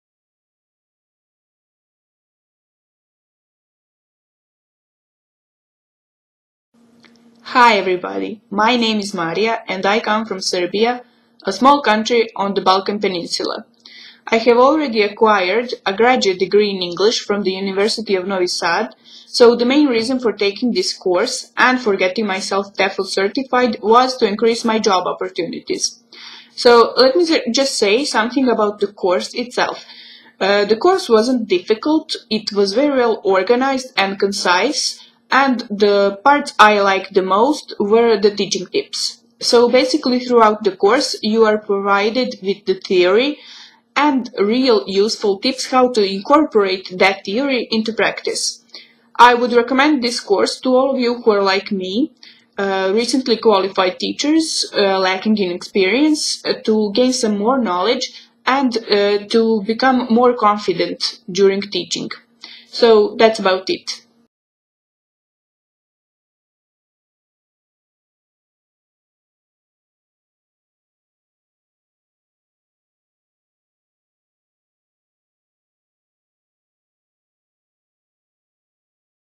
TESOL TEFL Reviews - Video Testimonial – Marija
Marija from Serbia, having completed a degree in English, wanted to take her TEFL course with ITTT to increase her employment opportunities. In this TEFL review, Marija tells us how she found the course to be very well organized and really appreciated the way the course showed her how to take the elements of teaching theory covered in the course and put them into practice. Marija recommends the course to anyone without teaching experience to enhance their knowledge of English language teaching and to gain confidence in the classroom.